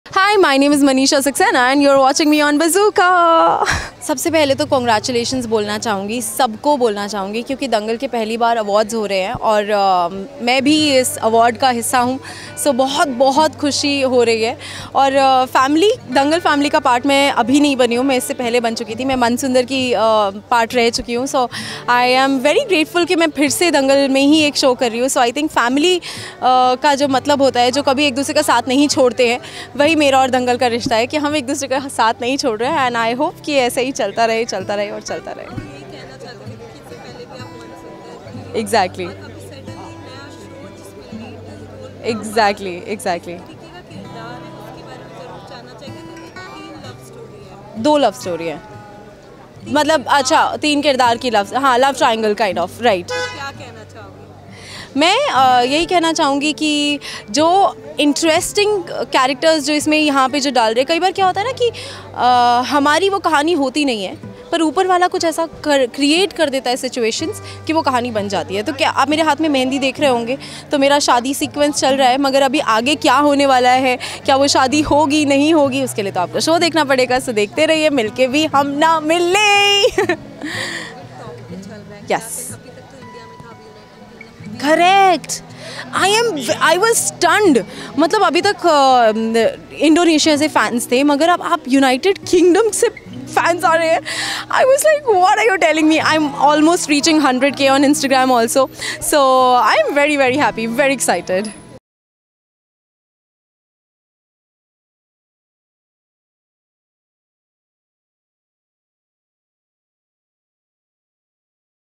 Don't Miss: Manisha Saxena Discusses Dangal Family Awards 2024 |Milke Bhi Hum Na Mile
Exclusive Interview - Manisha Saxena Talking About  Dangal Family Awards 2024 |Milke Bhi Hum Na Mile